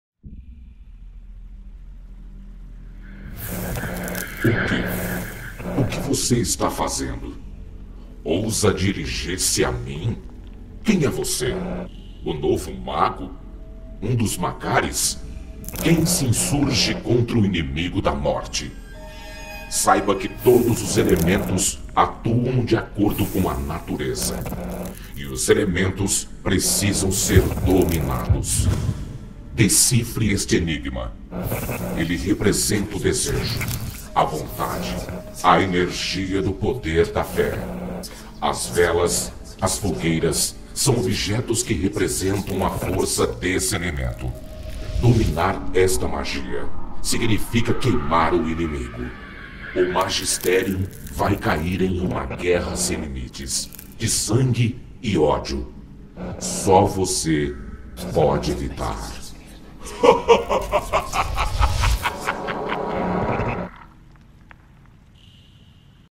Desafio Magisterium #3
Participantes do DesafioMagisterium, preparem-se: mais um enigma foi lançado pelo Inimigo da Morte. Será que um de vocês conseguirá conquistar este elemento? Descubra em breve.